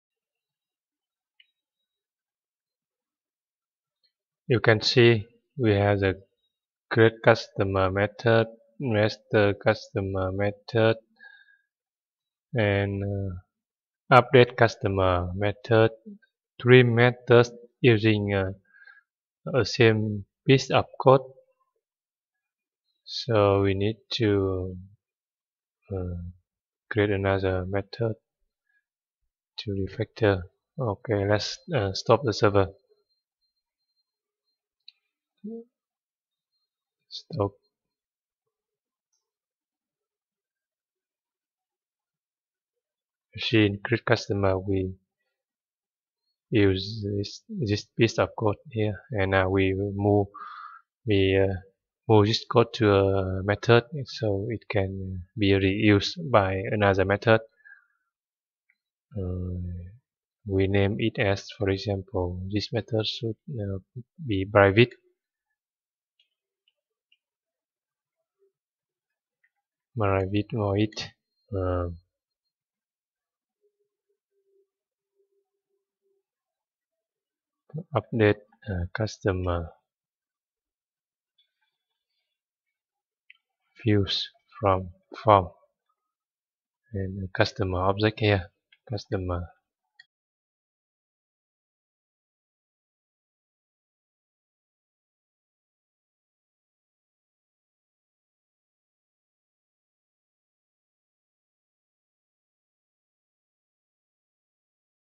Lecture 121 - Refactor and Test again
Refactor code for customer registration and test again.

This video is a part of the course "Java Servlet, JSP and Hibernate: Build a Complete Website".